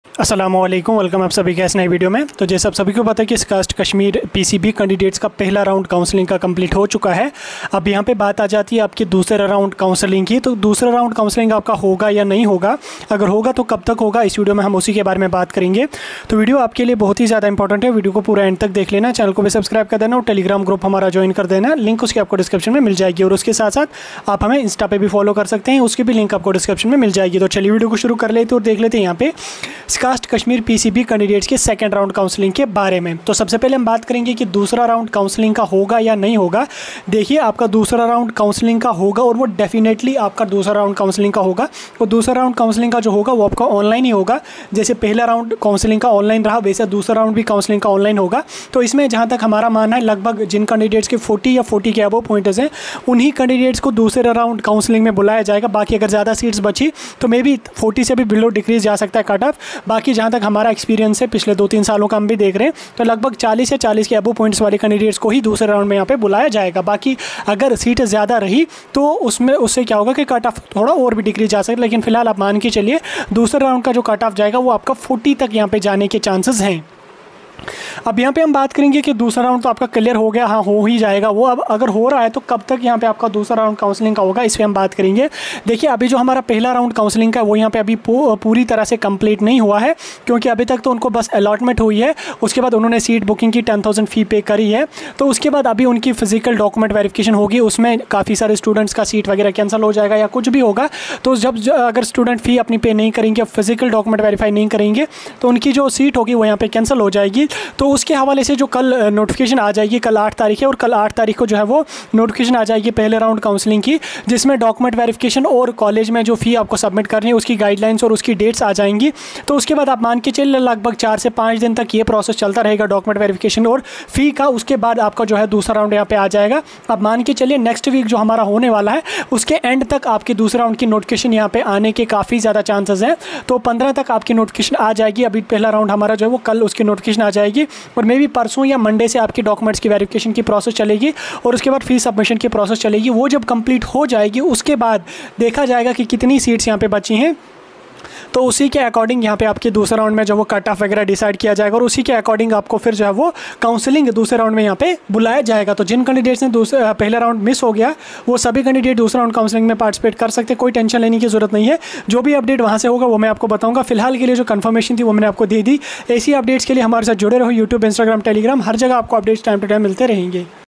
SKUAST Kashmir 2nd Round Councelling Update 🔥 Cutt Off & Tentative Dates Discussed In Video
Join Telegram Group For All Update